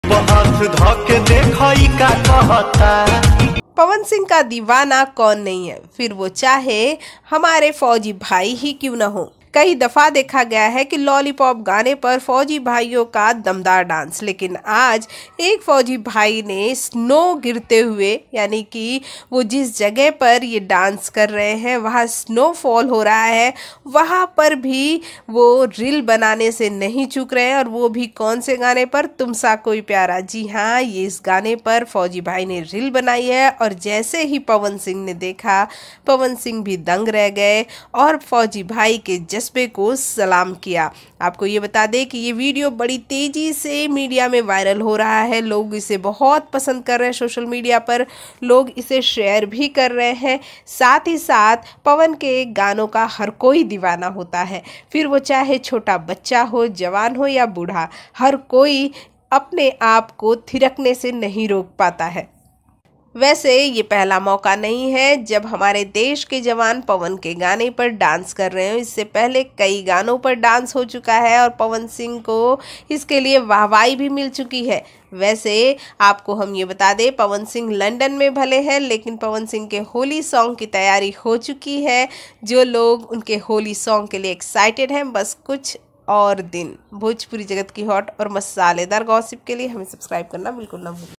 पवन सिंह के गाने पर जमकर डांस किया एक फ़ौजी ने l
पवनसिंह के गाने पर जमकर डांस किया एक फ़ौजी ने l